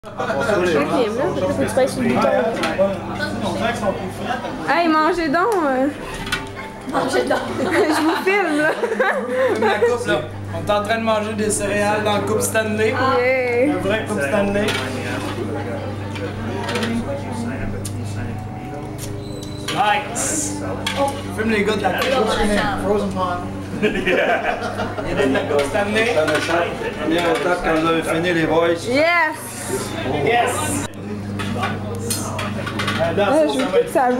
Mini Wheats cereals in the stanley cup !!!!
FX Allaire eats his breakfast in the Staley Cup... NICE !